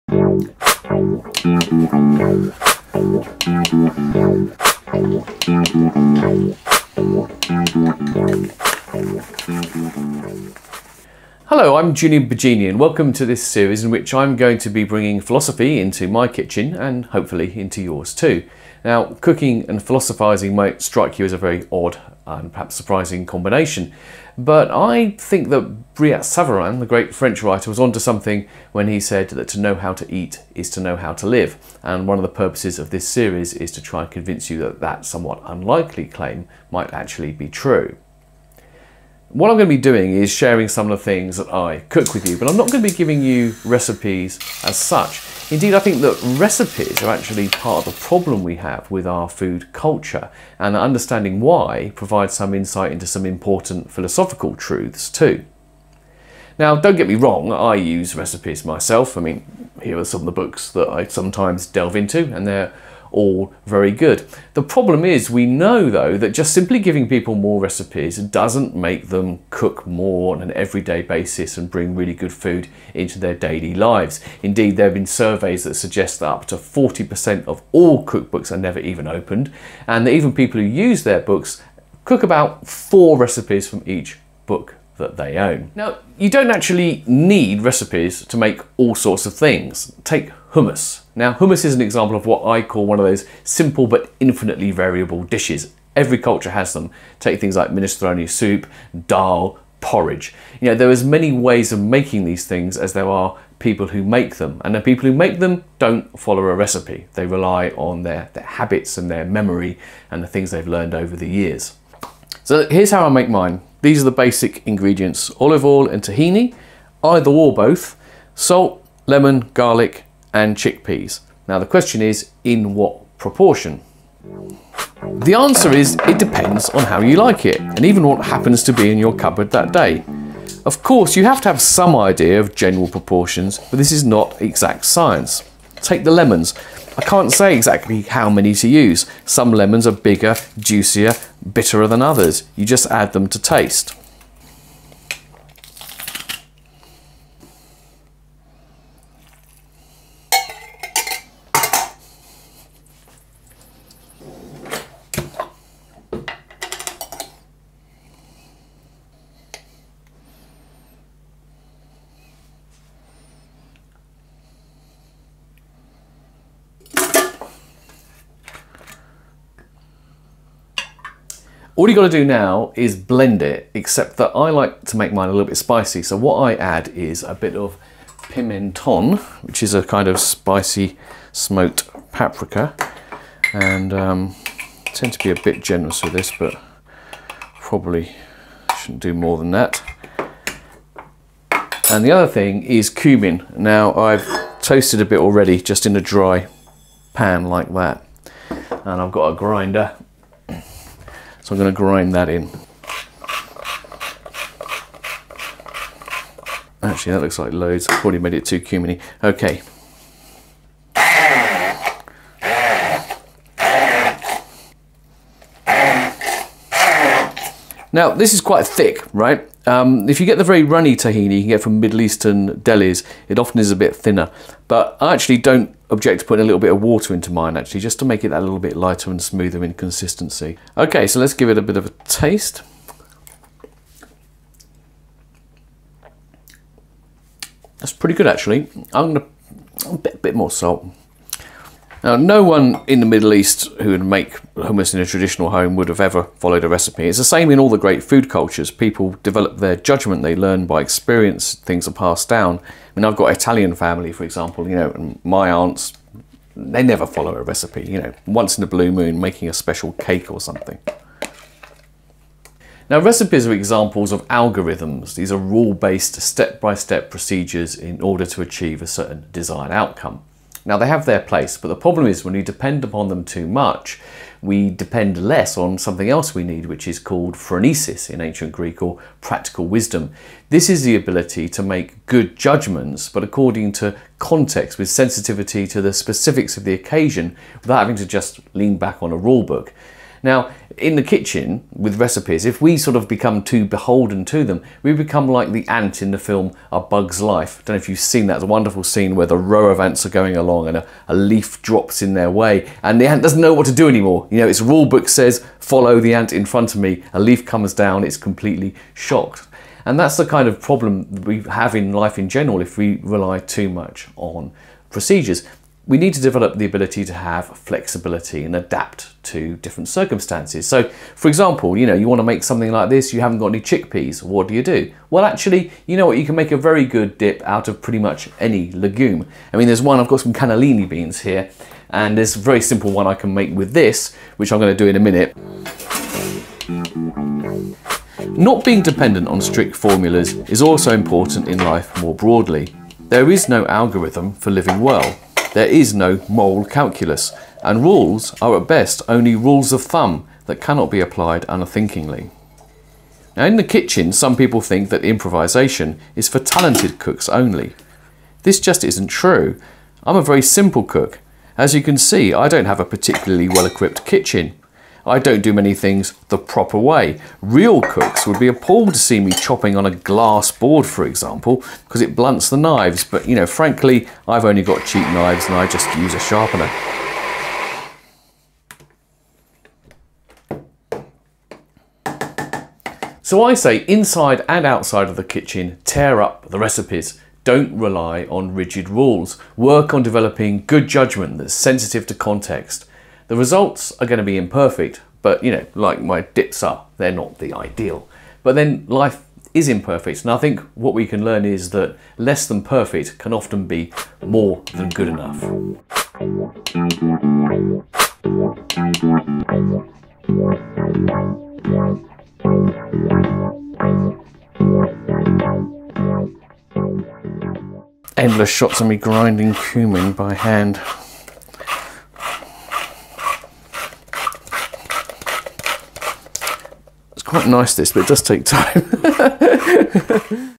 The Philosopher in the Kitchen (1)
Hummus and escaping the tyranny of recipes.
Filmed, Directed and Edited by George Miller.
Written by Julian Baggini.